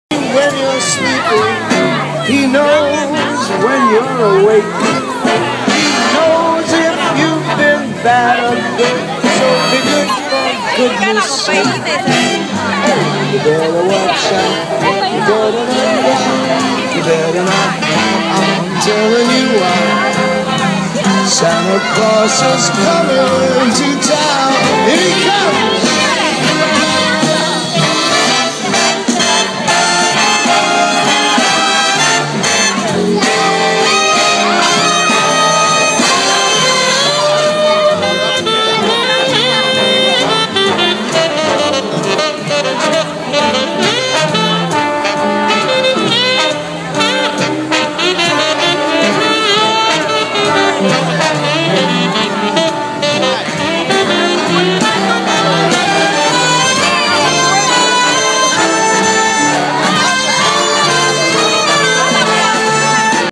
Fort Lee Christmas
Annual Christmas Celebration in Fort Lee. Mayor Sokolich and business partner Marc Macri  and Council join together with families to light the Christmas tree, and enjoy the fanciful festivities while Santa greets one and all. Beautiful  and joyous celebration held at the Jack Alter Community Center.